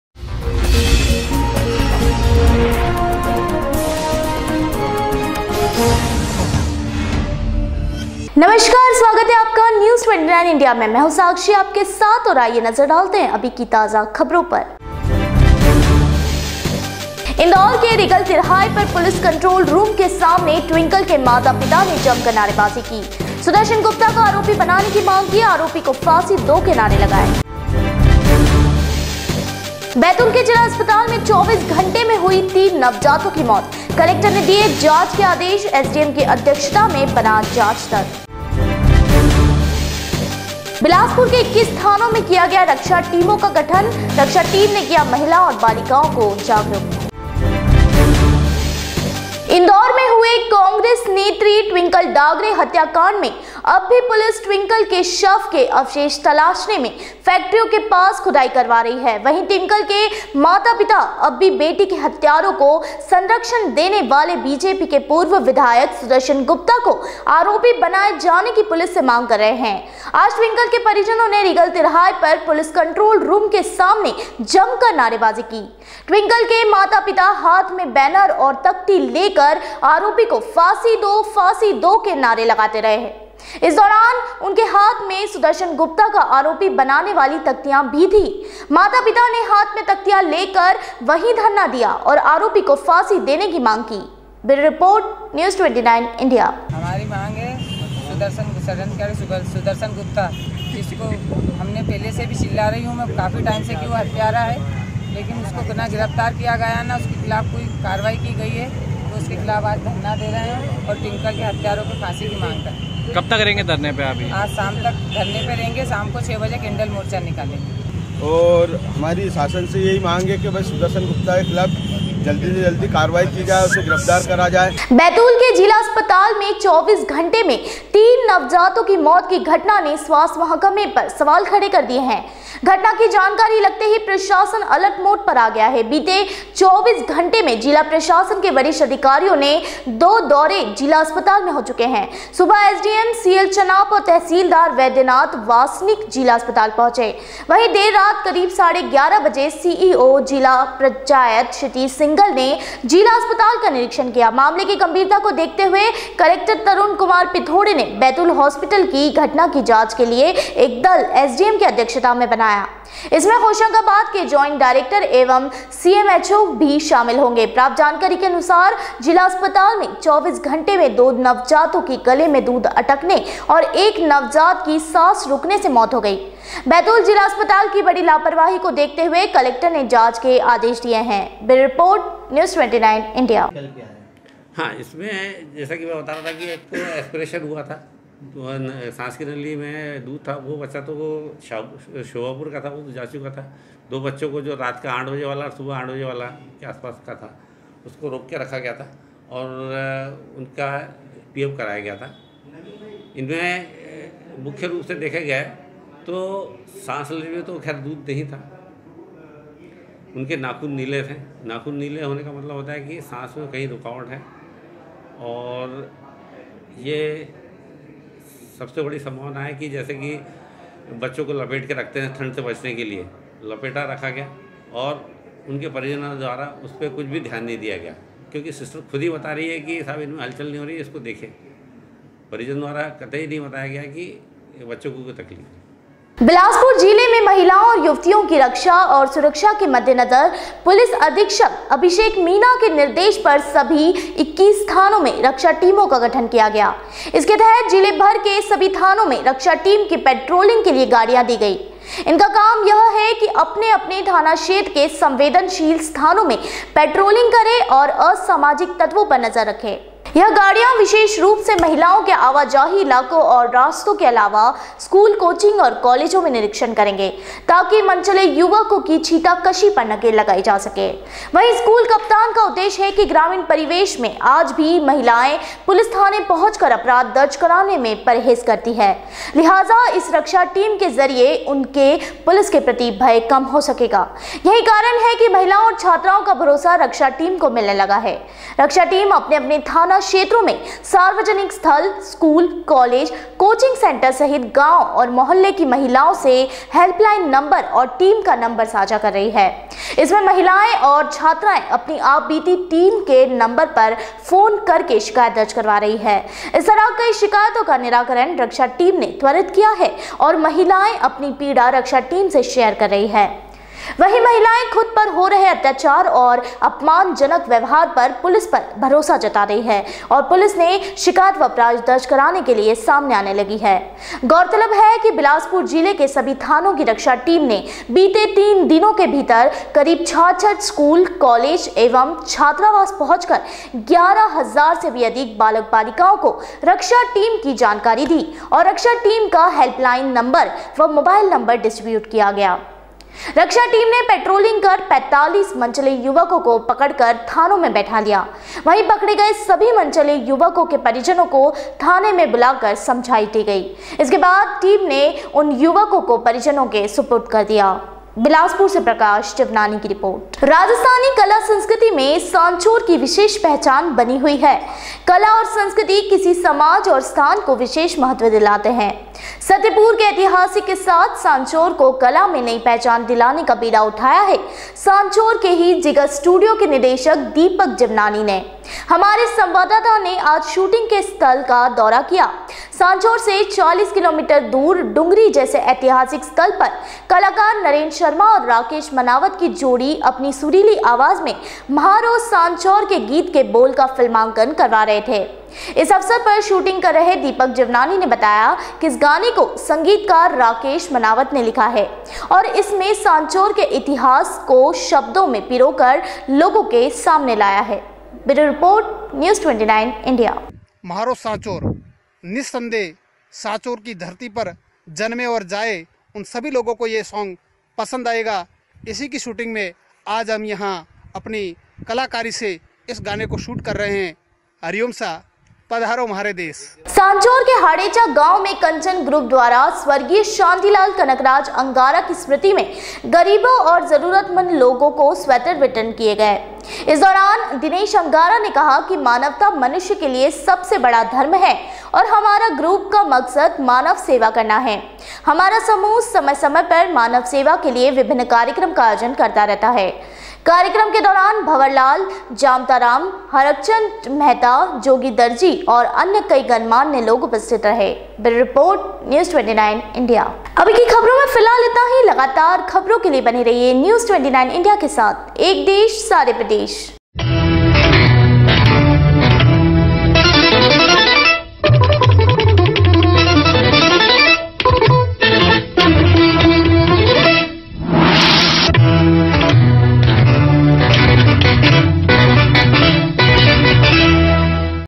News29India#Bulletin 2 feb lot 06 पुलिस कंट्रोल रूम के सामने ट्विंकल के माता पिता ने की नारेबाजी
1इंदौर के रीगल तिराहे पर पुलिस कंट्रोल रूम के सामने ट्विंकल के माता पिता ने की जमकर नारेबाजी, की सुदर्शन गुप्ता को आरोपी बनाने की मांग, आरोपी को फांसी दो के लगाए नारे
2 बैतूल के जिला अस्पताल में 24 घंटे में हुई 3 नवजातों की मौत, कलेक्टर ने दिए जांच के आदेश, एडीएम की अध्यक्षता में बना जांच दल,
3 बिलासपुर के 21 थानों में किया गया रक्षा टीमों का गठन किया गया, रक्षा टीम ने किया महिलाओं और बालिकाओं को जागरूक